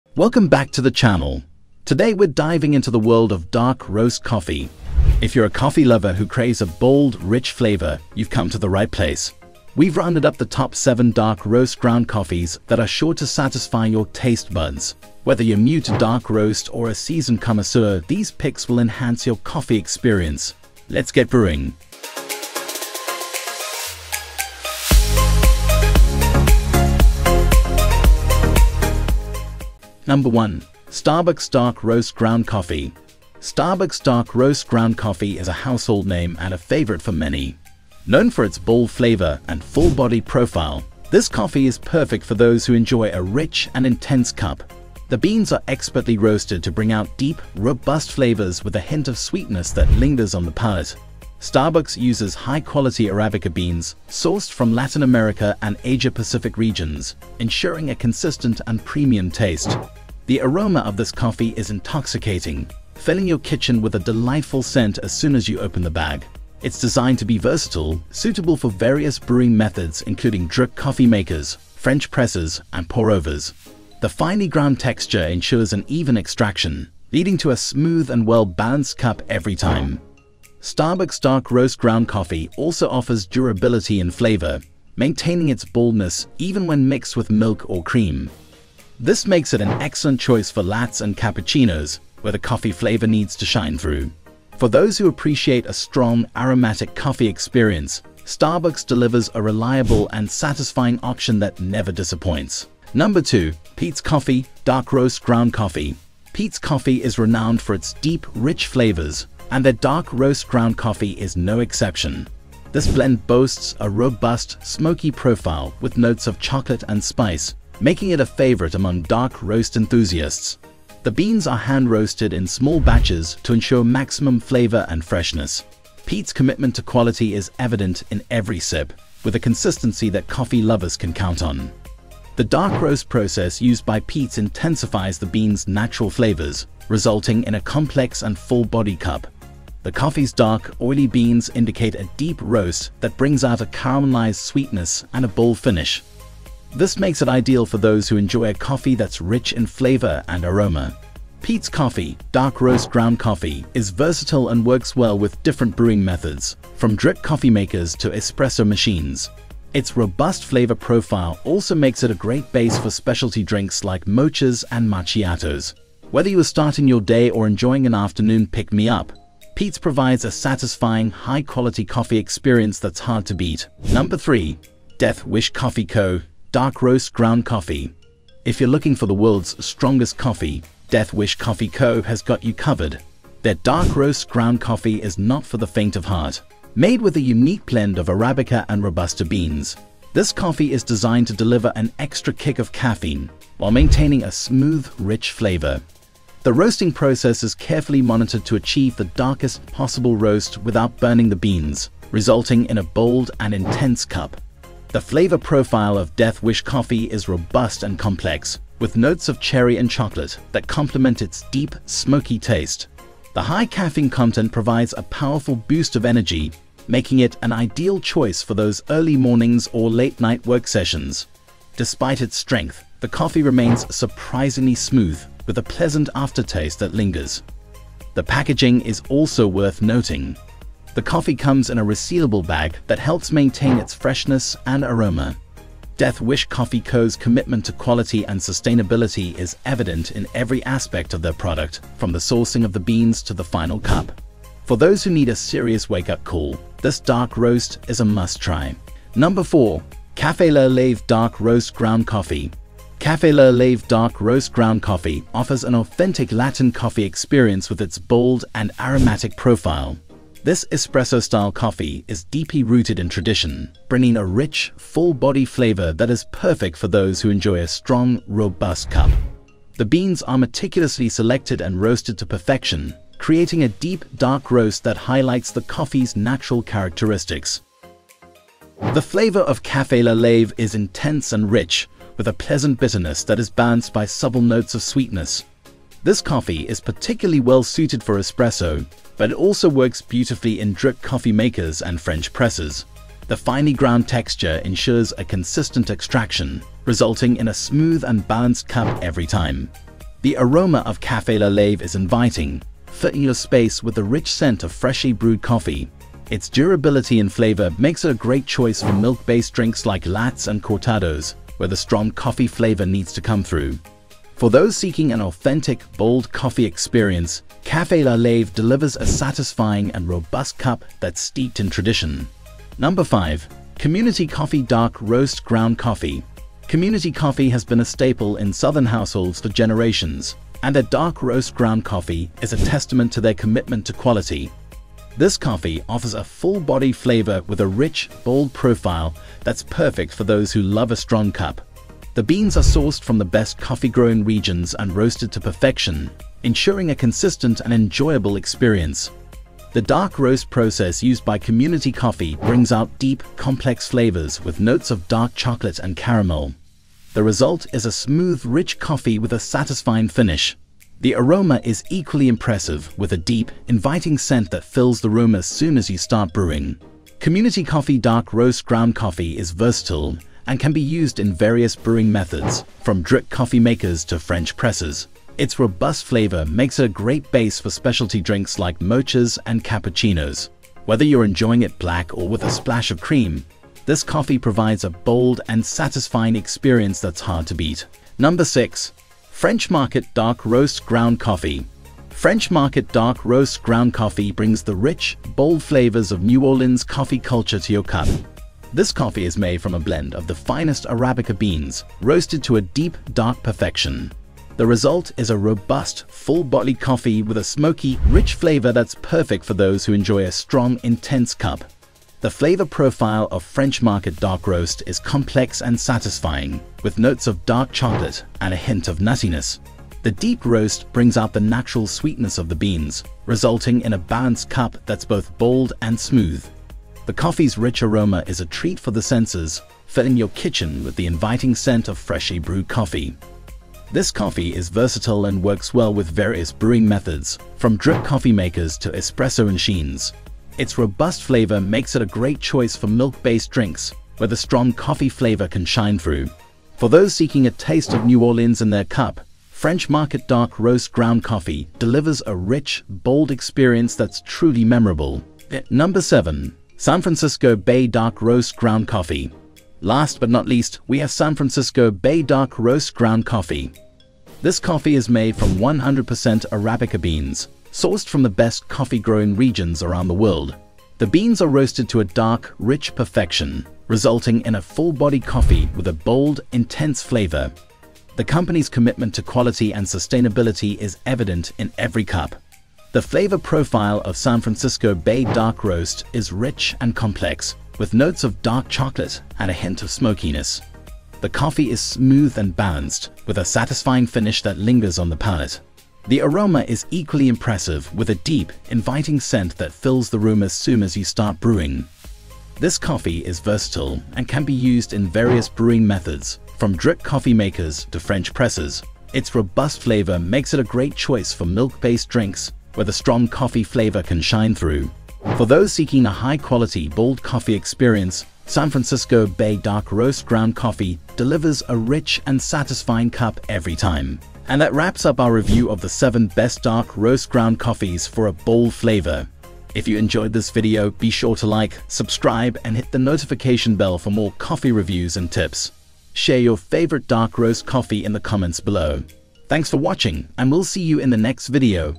7 Best Dark Roast Ground Coffee for a Bold Flavor
Top 7 Best Dark Roast Ground Coffees in 2024

Links to the Best Dark Roast Ground Coffees we listed in today's Dark Roast Ground Coffee review video:

1 . Starbucks Dark Roast Ground Coffee
2 . Peet's Coffee, Dark Roast Ground Coffee
3 . Death Wish Coffee Co., Dark Roast Ground Coffee
4 . Cafe La Llave Dark Roast Ground Coffee
5 . Community Coffee Dark Roast Ground Coffee
6 . French Market Dark Roast Ground Coffee
7 . San Francisco Bay Dark Roast Ground Coffee

What are the Best Dark Roast Ground Coffees in 2024?

In today's video we reviewed the Top 7 Best Dark Roast Ground Coffees on the market in 2024. We made this list based on our personal opinion and we ranked them in no particular order, after doing our research based on their prices, quality, durability, brand reputation and many more.

After watching this video you will know which are the best budget, top selling and top rated Dark Roast Ground Coffee on the market today.

If you choose from this list you can be sure that you buy one of the best Dark Roast Ground Coffees available today.